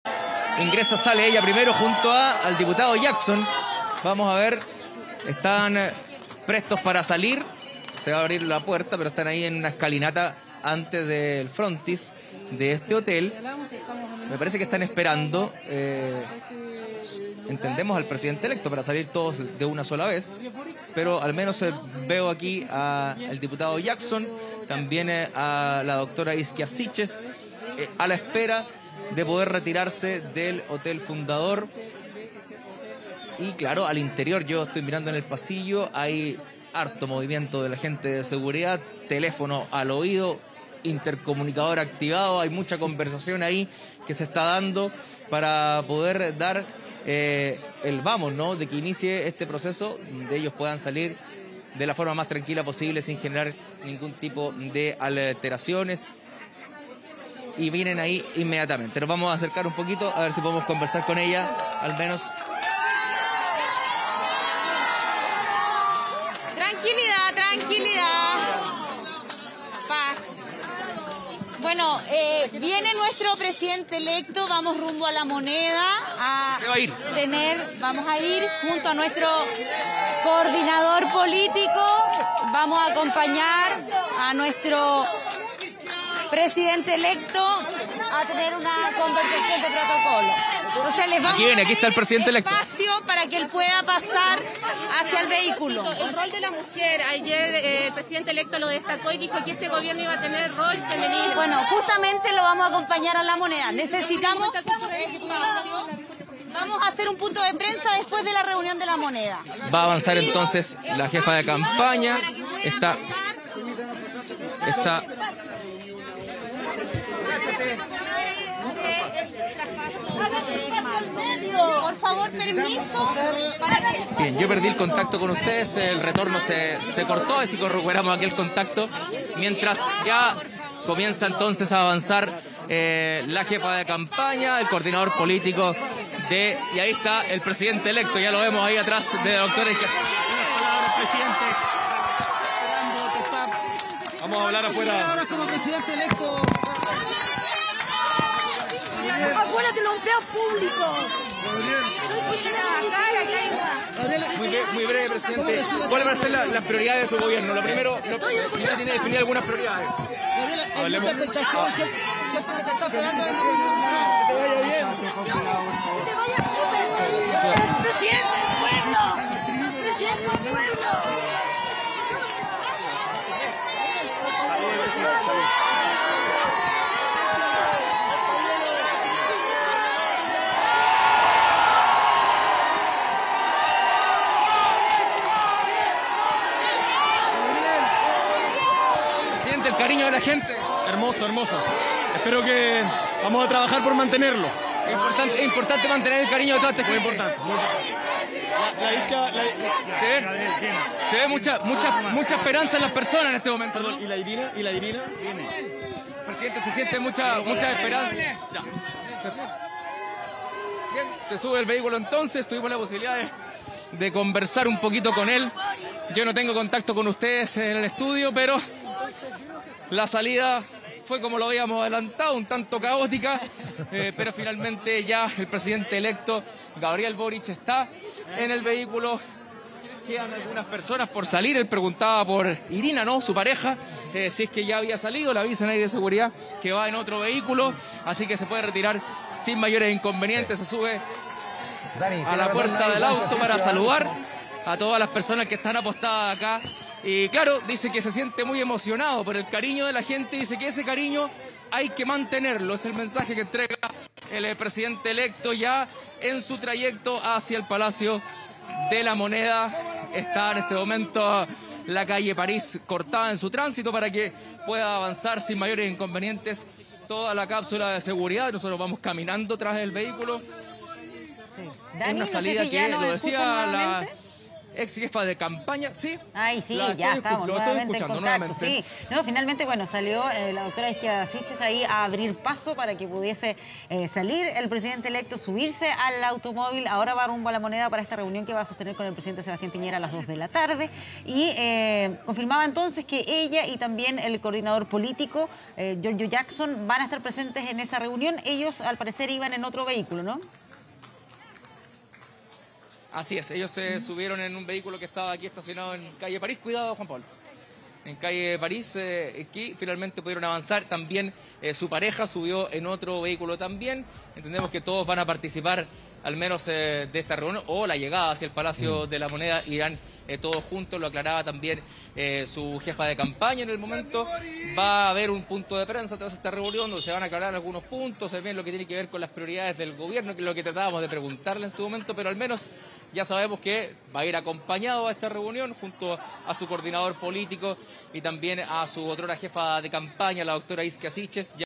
Gabriel Boric llega a La Moneda para tener reunión junto a Sebastián Piñera
El presidente electo llegó junto a Izkia Siches y Giorgio Jackson para reunirse con el actual jefe de Estado.